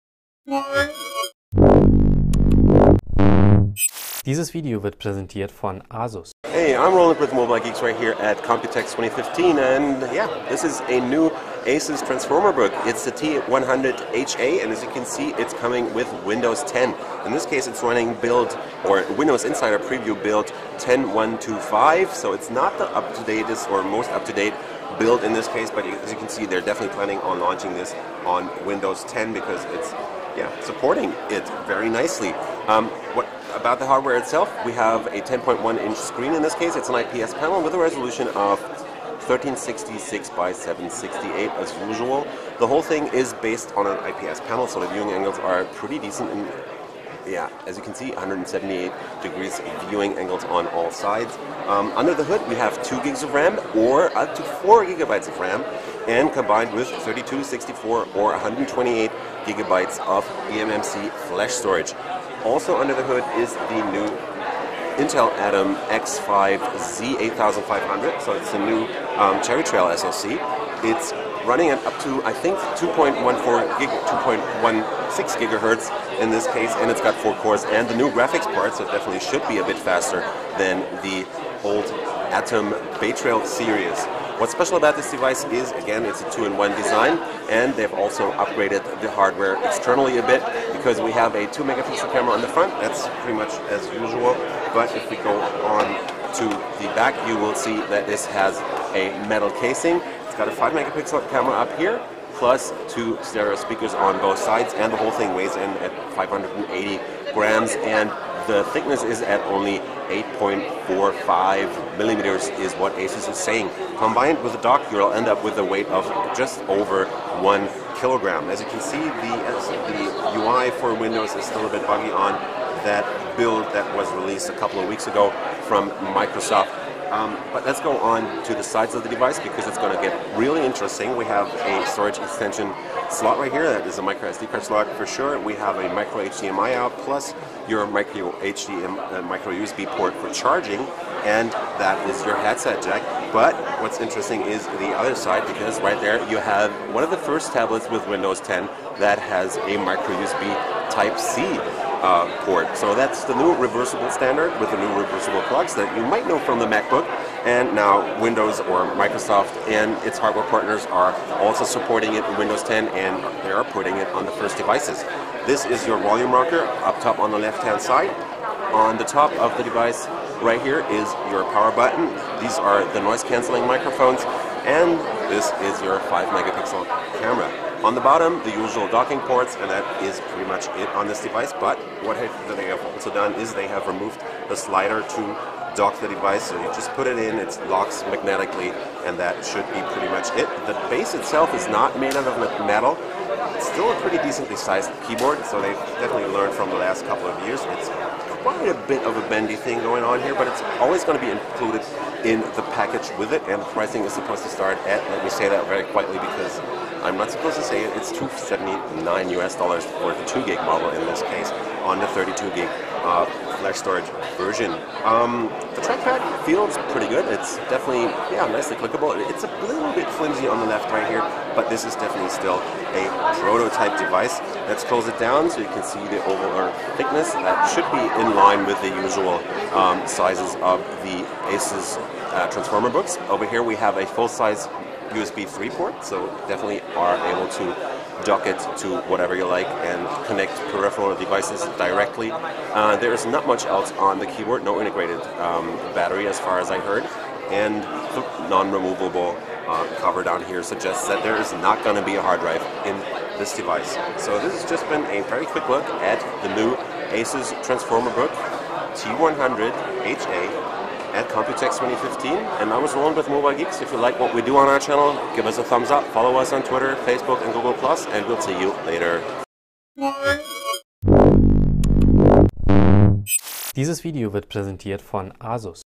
ASUS Transformer Book T100HA - First Windows 10 tablet hands on
ASUS has launched the first tablet and 2-in-1 notebook running Windows 10 at Computex 2015 - the ASUS Transformer Book T100HA that is based on the new Intel Atom x5-z8500 quad core SoC running at up to 2,4 ghz, including 2 or up to 4 GB RAM and eMMC-SSDs with 32, 64 or 128 GB.